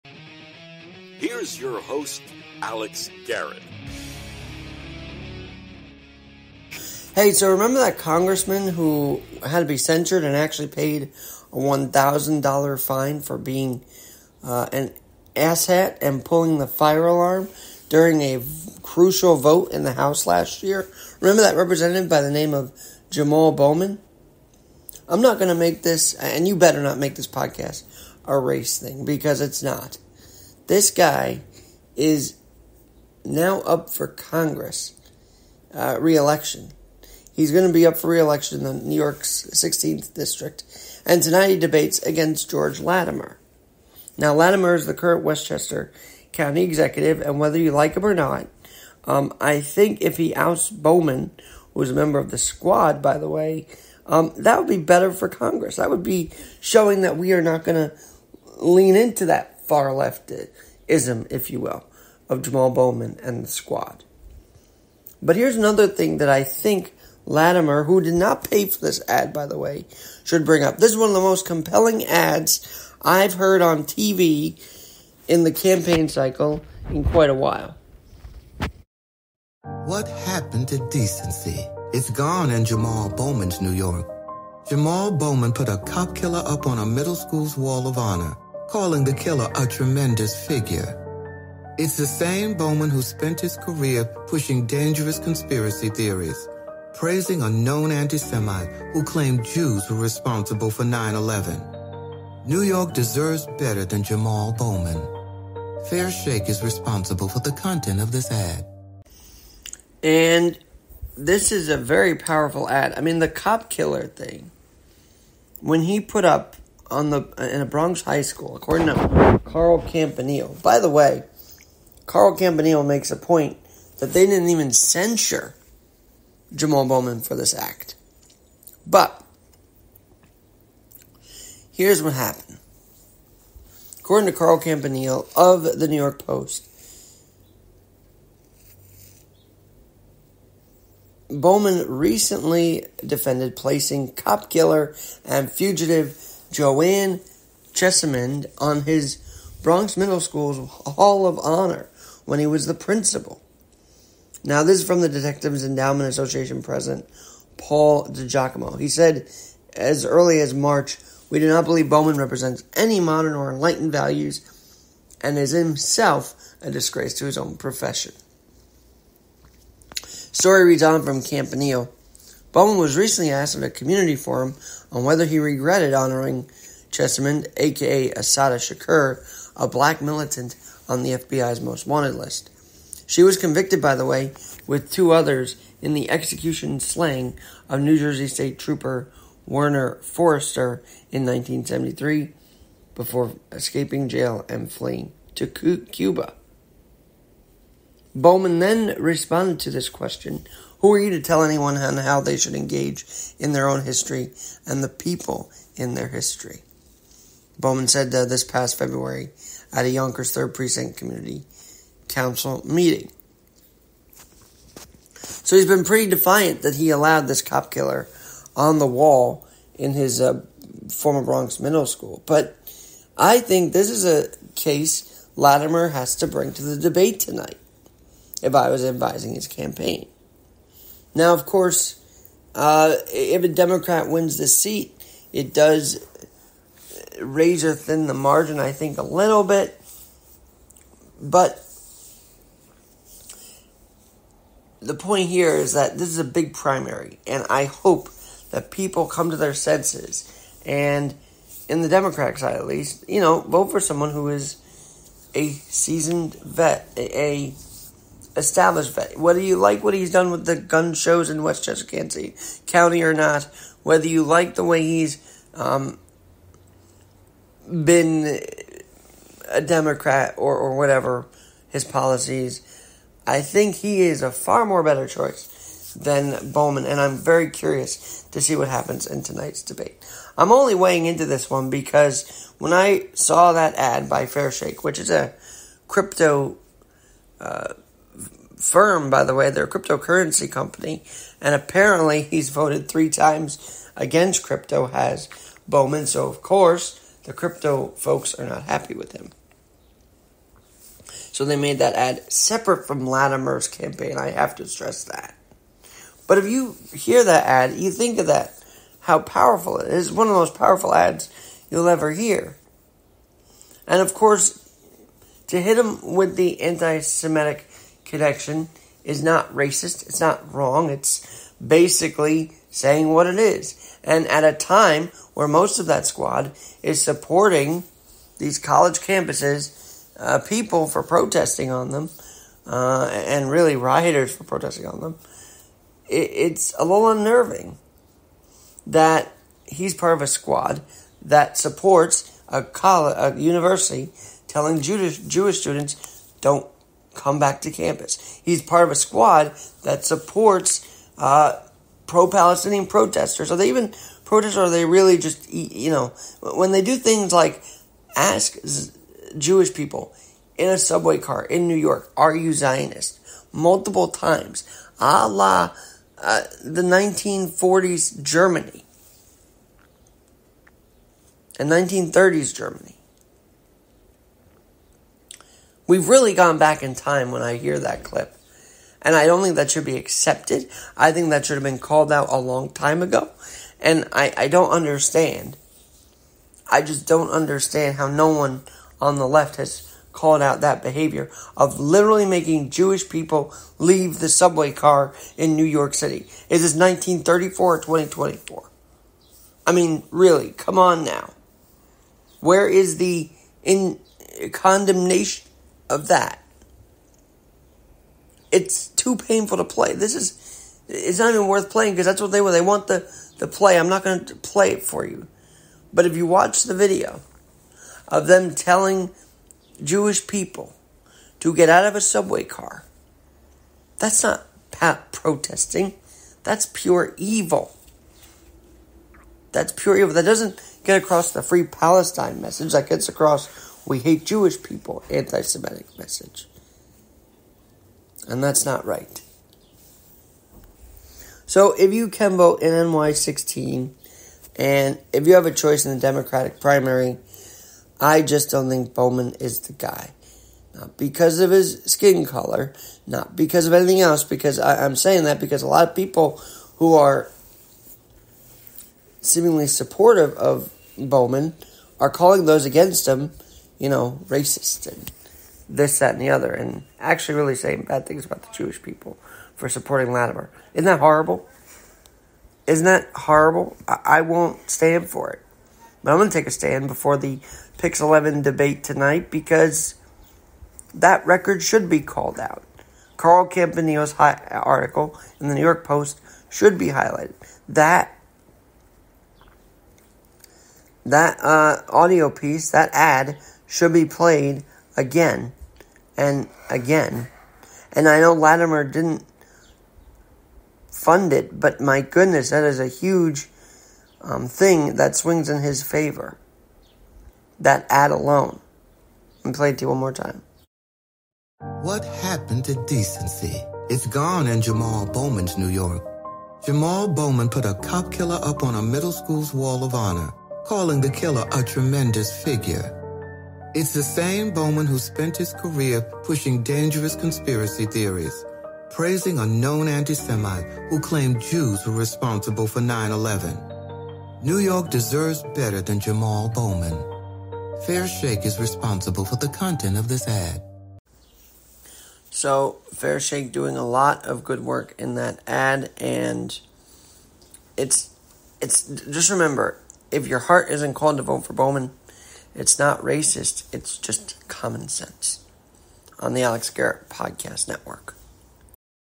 What to Know About George Latimer vs. Rep. Jamaal Bowman in NY-16 6-18-24
Ahead of NY-16 debate on Pix 11, why the FairShake PAC’s ad against Rep. Jamaal Bowman is so powerful and FACTUAL. Also, why George Latimer should utilize this from the NYPost against Rep. Bowman in tonight’s debate on Pix 11